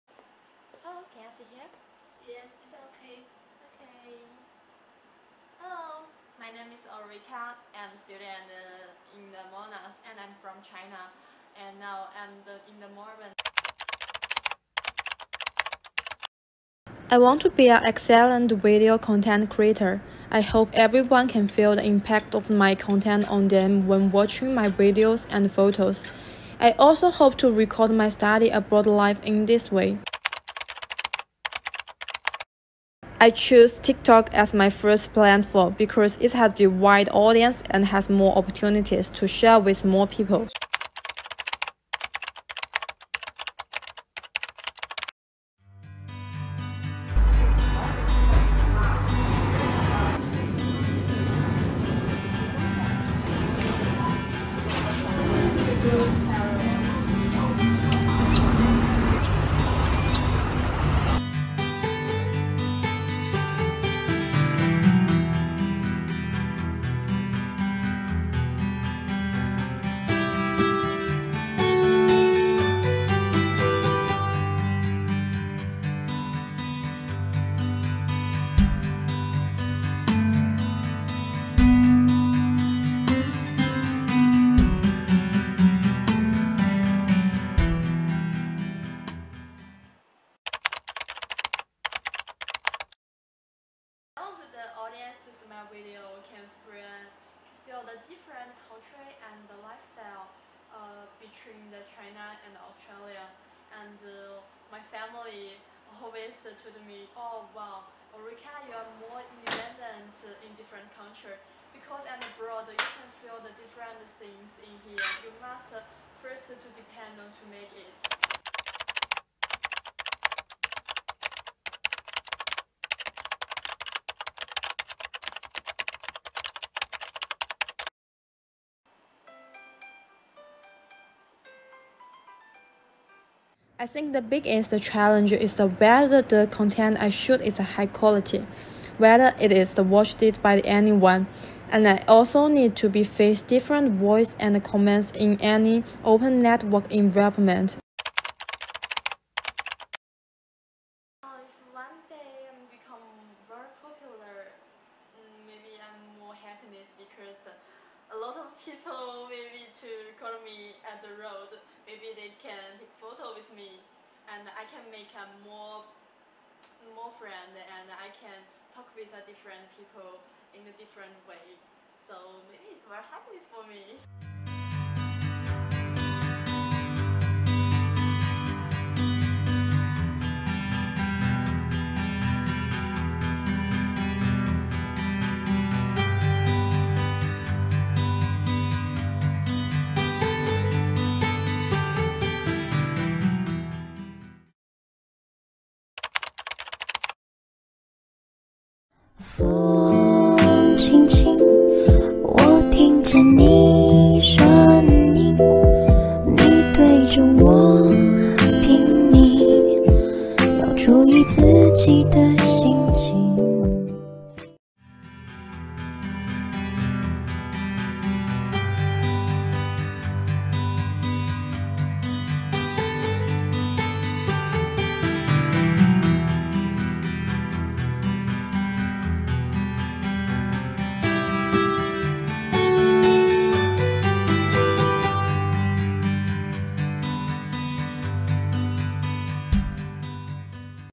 Xinyu Liu Mini documentary Dream
This mini-documentary tells an inspirational story. Ulrica, a Monash University student, has a dream, which is becoming an excellent video content creator, and she chooses TikTok as her first platform. Although she may encounter challenges in the process of achieving dream, the future will be more exciting.